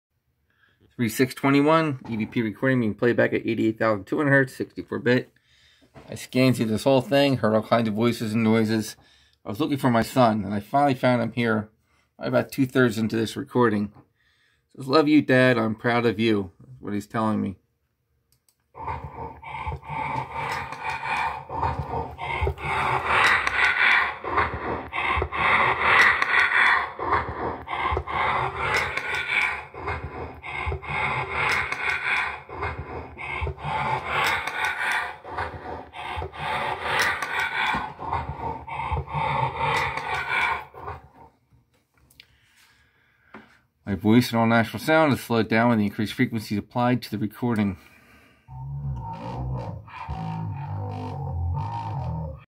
6. My son Jeremy’s EVP is finally heard saying “Love you Dad, I’m proud of you”
My son Jeremy’s EVP is finally heard saying “Love you Dad, I’m proud of you”